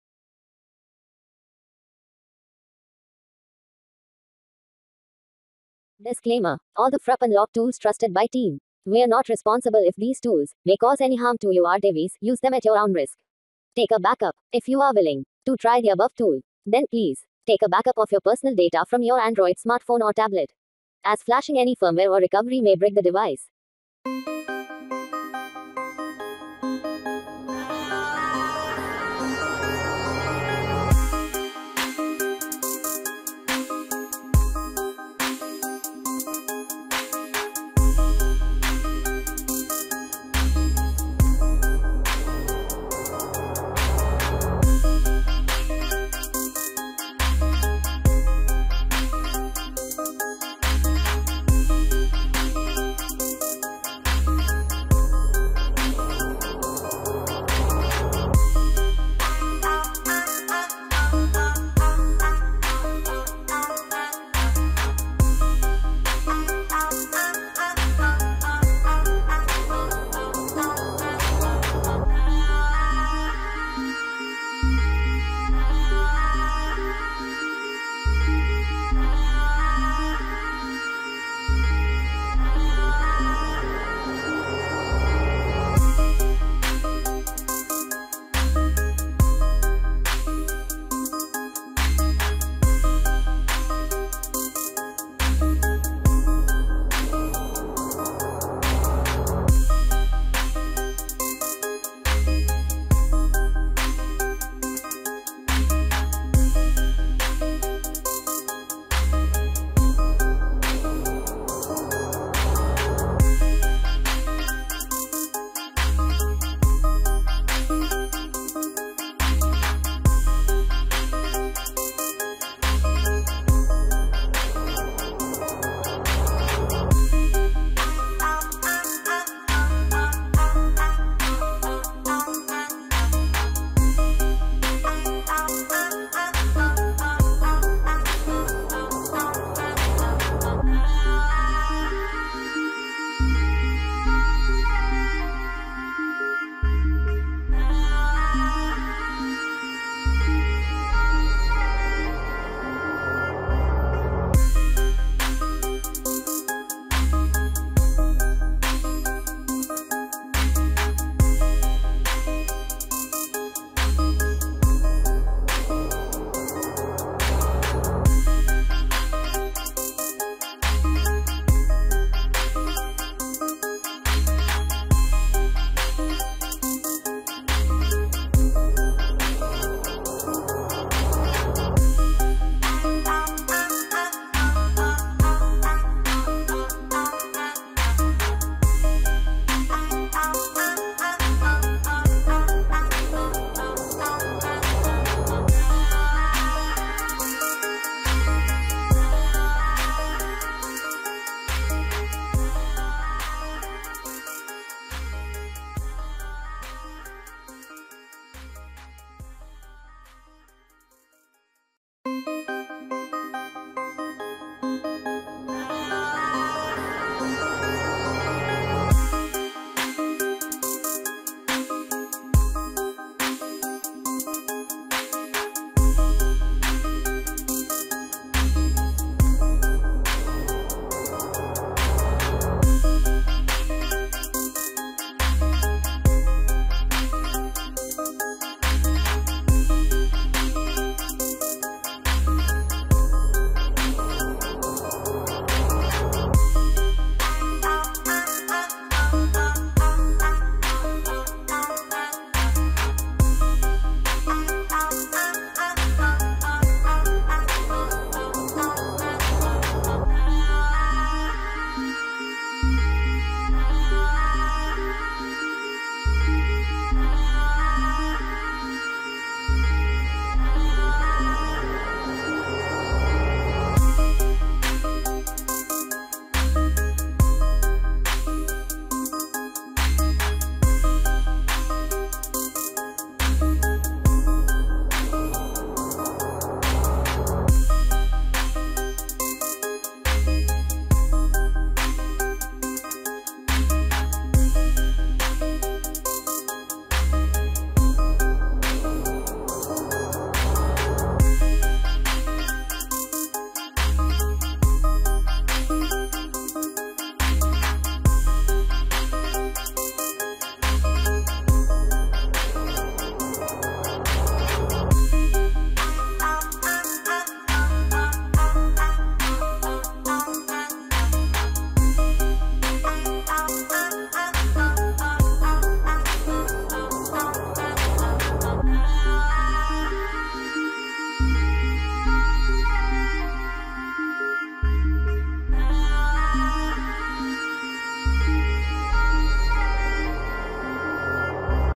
🔥 HASNIT3CH RAMDISK Tool V3 | Ramdisk Passcode & HelloScreen Bypass iOS15
Hello Screen Mode
- Read Hello
- Ramdisk 1
- Ramdisk 2
- Get File
- Check SSH
- Activate
Passcode Mode
- Read DFU
- Ramdisk 1
- Ramdisk 2
- Check SSH
- Backup
- Restore

Other Tools

- Purple Mode
- Hide Account
- Change SN

Changelog!!!

[ FreeDay 😍 ]

Welcome to FreeDay By HASNIT3CH Ramdisk

🔥 HASNIT3CH RAMDISK Tool | Ramdisk Passcode & HelloScreen Bypass iOS15
❇️ Generate Activation Data File Hello screen iOS15 In Dfu Mode
❇️ Compatible iOS11-15 Passcode iCloud Bypass(With Signal)✅
❇️ iOS15 Hello Screen iCloud Lock Bypass(Without Signal)❌
❇️ 7 to X Supported A10/A11 Cpu
❇️ Direct Boot Purple Mode In Windows A10/A11 Cpu
❇️ Auto Fix Apple Driver

To register your device for FREE, login to this free credits account:
by @HasniT3ch

Username:

Hasni-Free24

Password:

Hasni-Free